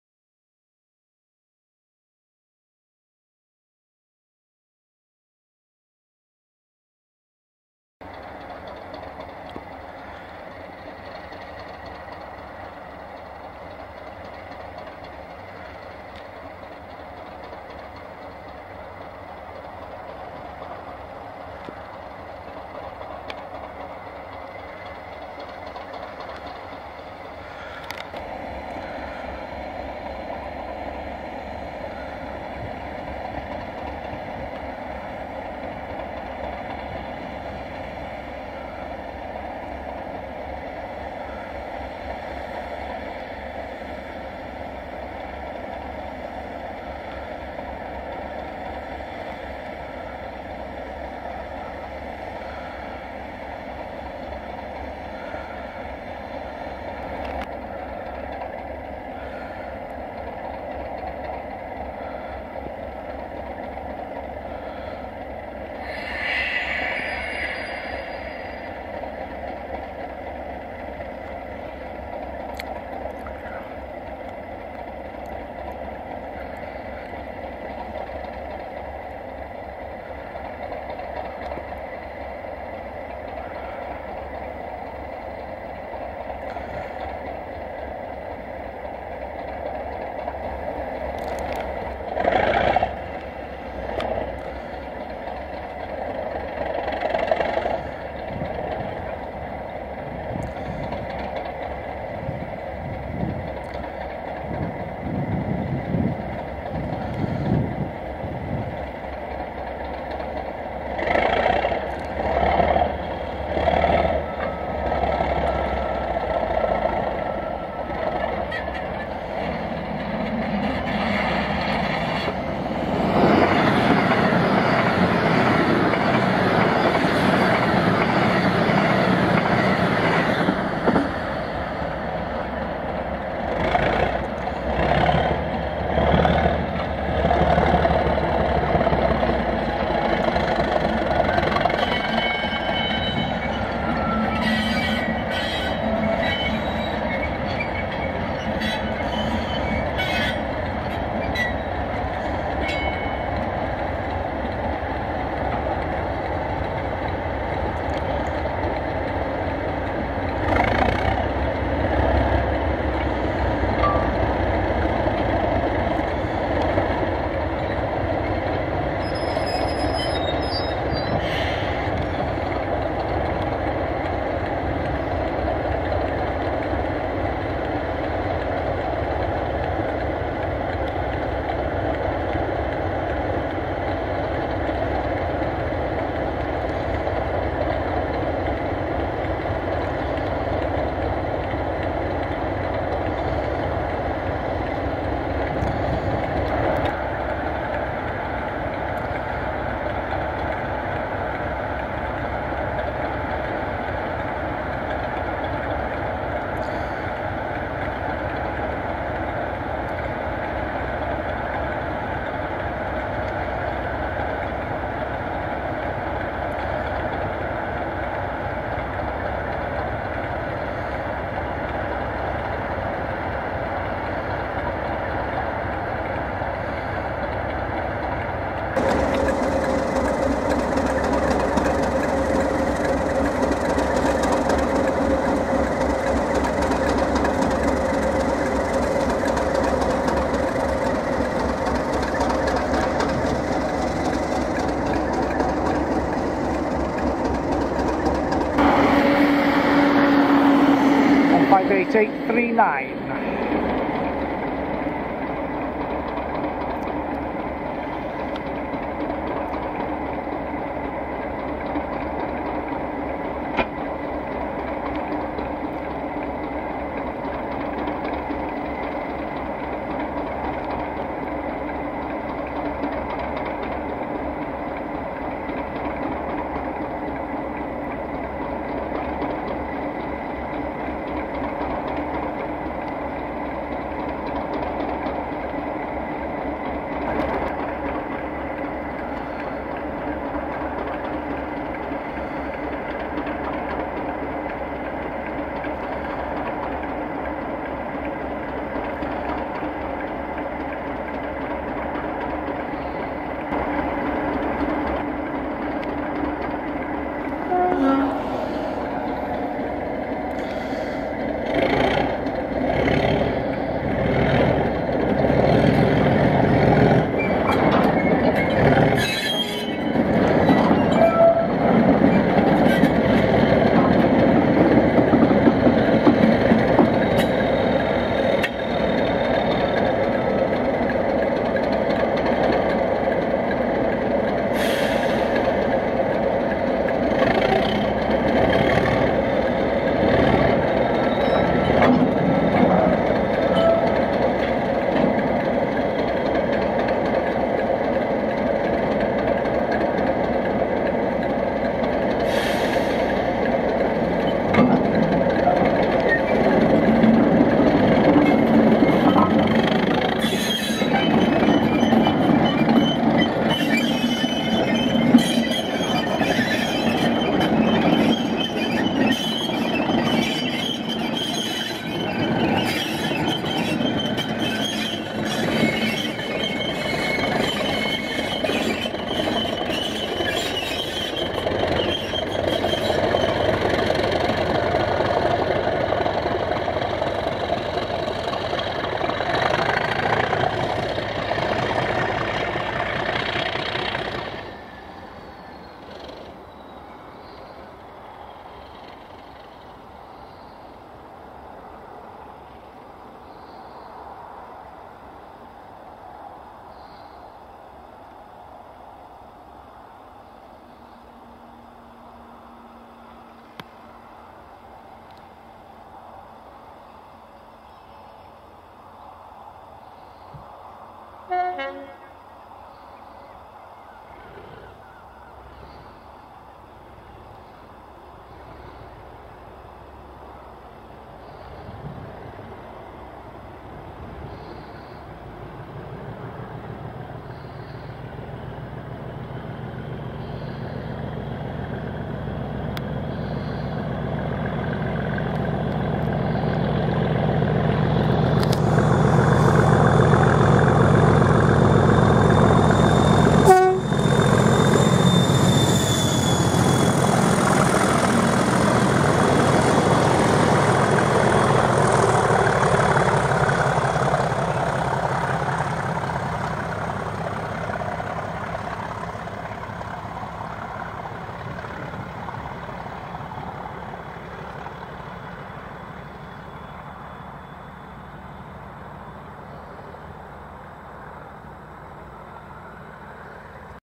DRS Class 37s 37059+37609 at Valley, Anglesey on 6K41 nuclear flasks to Crewe, 04-12-2014.
Thursday, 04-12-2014, at Valley triangle. 37059+37609 have run-round their two flasks on the triangle, and are shown waiting for access to the main line, then propelling onto it, followed by a volcanic departure for Crewe.
Apologies for camera-shake, and some heavy breathing, caused by my having rushed to the triangle hoping to catch them actually running-round.